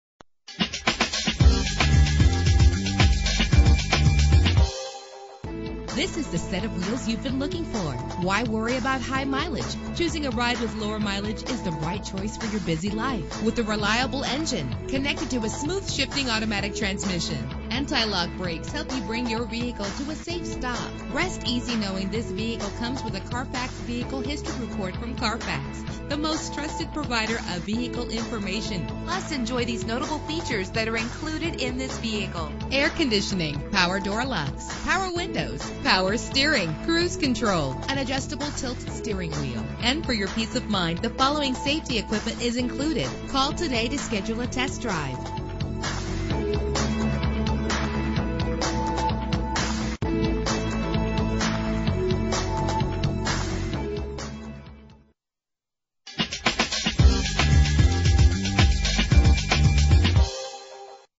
2008 Dodge Ram 1500 Quad Cab - Phoenix AZ
Year: 2008
Make: Dodge
Model: Ram 1500 Quad Cab
Trim: ST Pickup 4D 6 1/4 ft
Engine: 8 cylinder 4.7 Liter
Transmission:
Color: Light Graystone Pearl
Mileage: 14918
Address:
2425 West Camelback Road